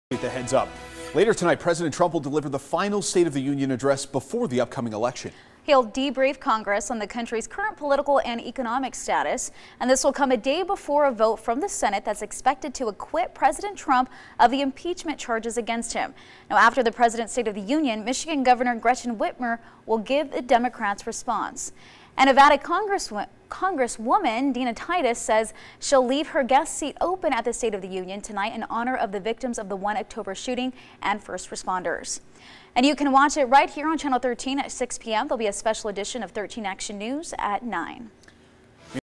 President Donald Trump to deliver State of the Union on Tuesday
President Donald Trump is set to deliver the State of the Union address in the Capitol Building on Tuesday evening. Nevada congresswoman Dina Titus is using her guest seat at the event to honor the victims of the 1 October shooting.   You can watch the address tonight at 6 p.m. on Channel 13. We will have a special edition of Action News at 9 p.m.